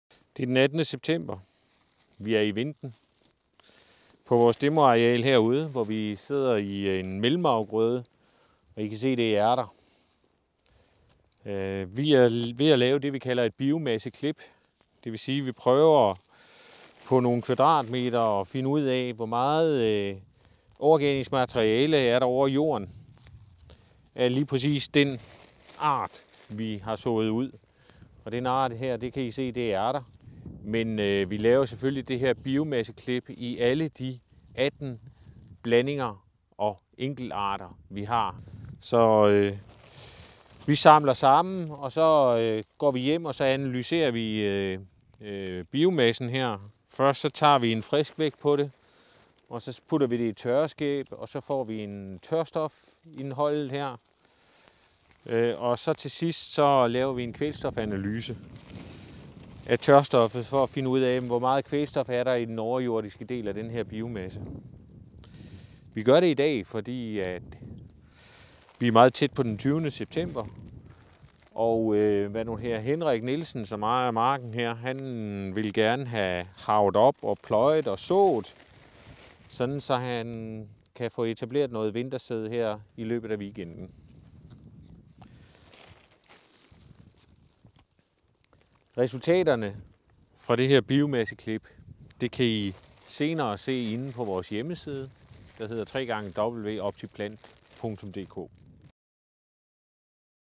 Biomasseklip af mellemafgrøden ved Vinten (OptiPlant)
I både mellem- og efterafgrøderne på demonstrationsarealerne ved Vinten og på Alrø tager vi biomasseklip for at bestemme tilvæksten målt som tørstofindhold og kvælstofindhold. Når prøverne er analyseret kan resultaterne ses på www. OptiPlant.dk.